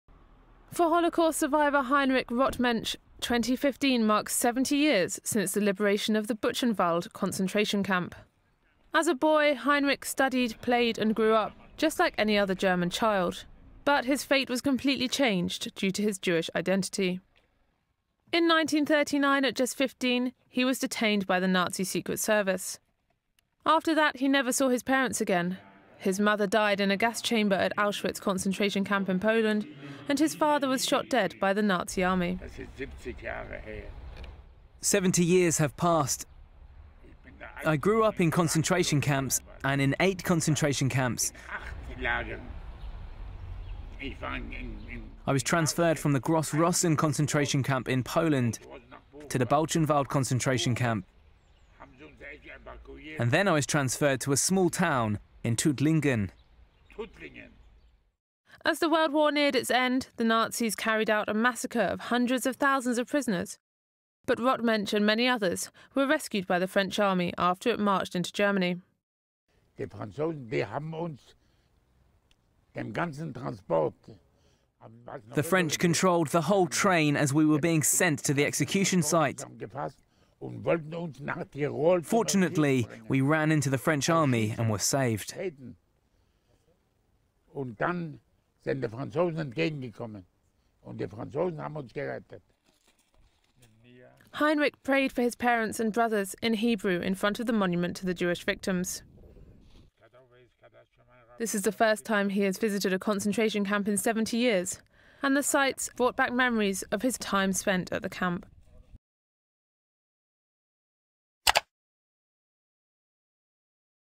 Holocaust survivor revisits Buchenwald concentration camp
A Holocaust survivor has revisited Buchenwald concentration camp, 70 years on from the camp's liberation . Report by Hettie Maylam.

More stories from ODN:

Costa Rica goes 75 days powering itself using only renewable energy